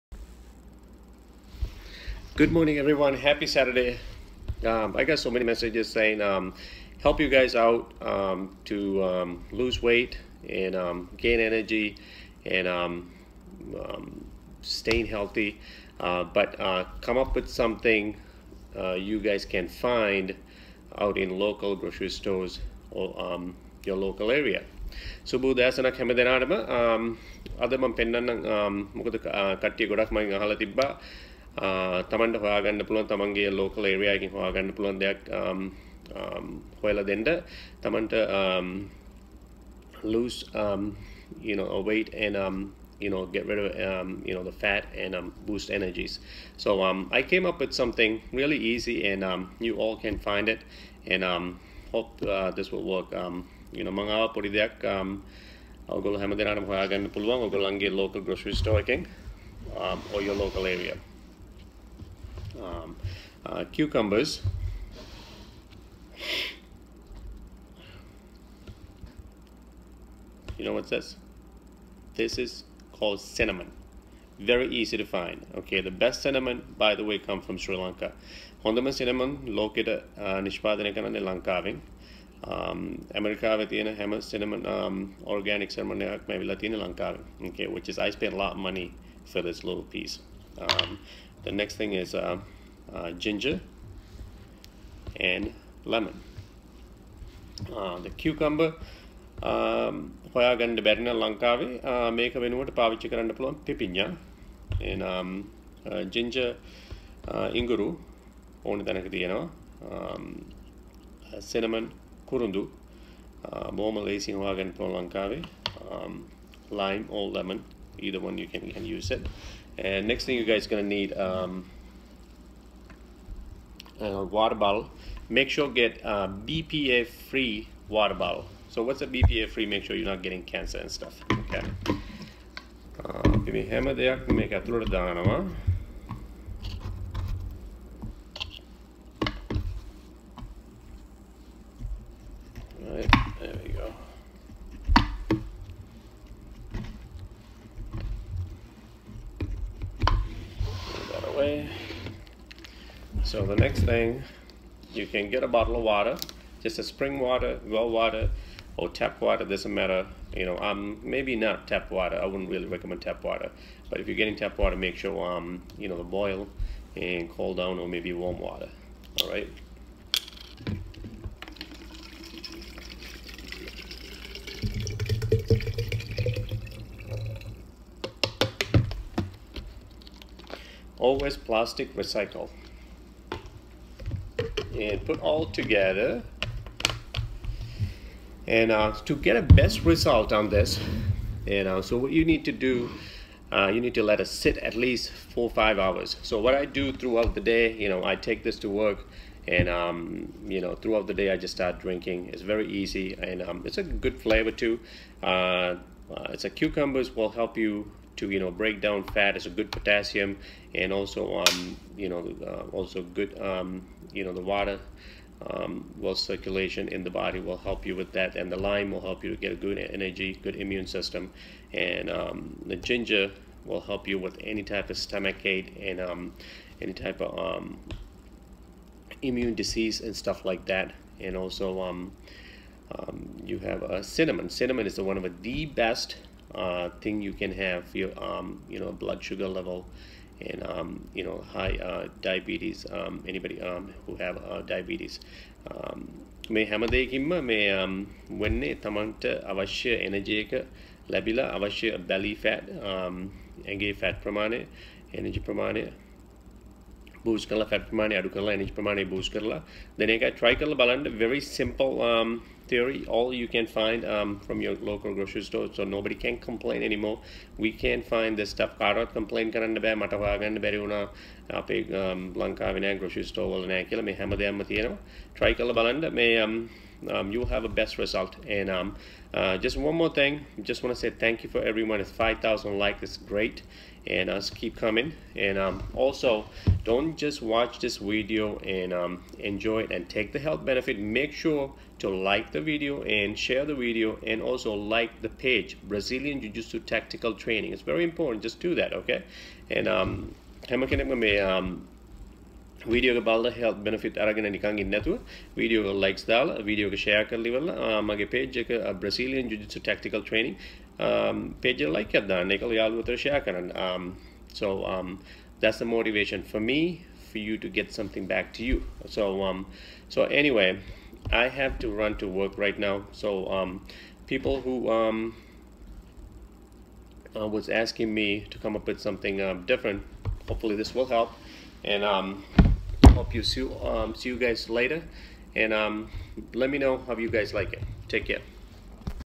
Fat bust fast with Ginger-Lemon-Cucumber-Cinnamon
Try this remedy to stay healthy and lose fat .. Ginger -lemon- cucumber- and cinnamon mix with water .. enjoy 🇺🇸❤️👊🏽✅